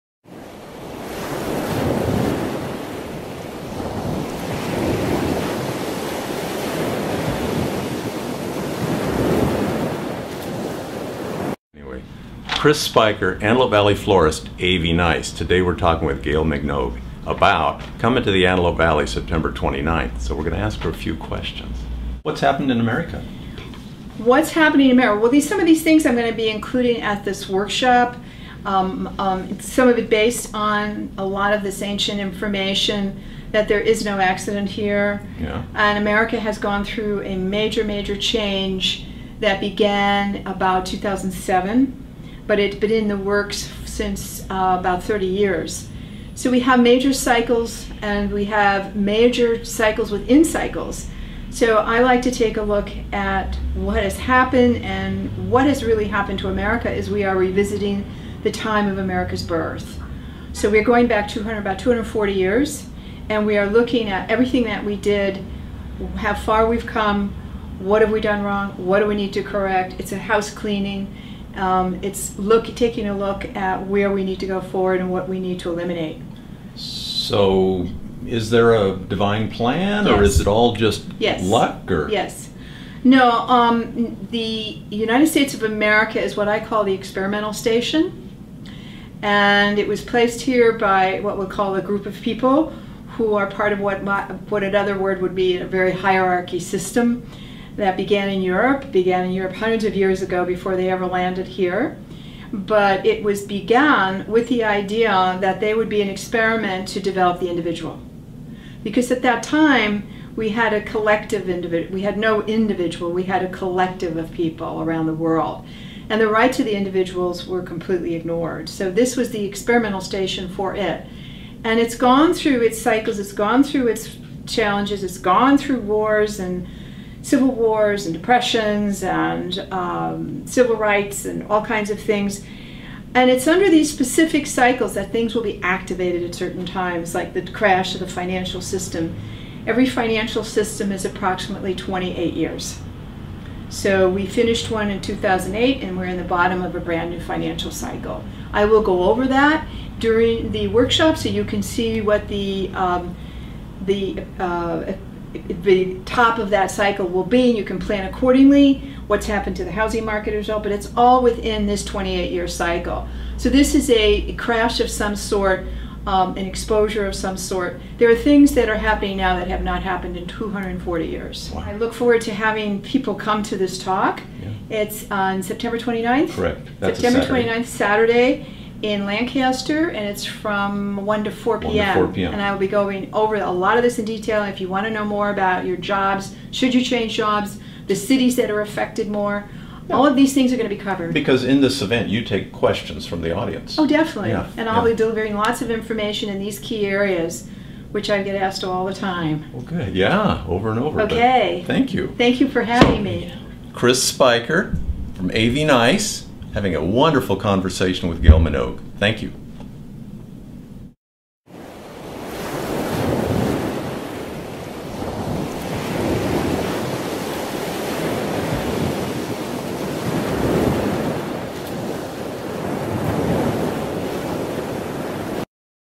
Gail Minogue speaks in Lancaster on trends and analysis 2012
Gail spoke with us, she came to Lancaster and we got the chance to interview her.  We covered a wide range of topics, go to her website for more info.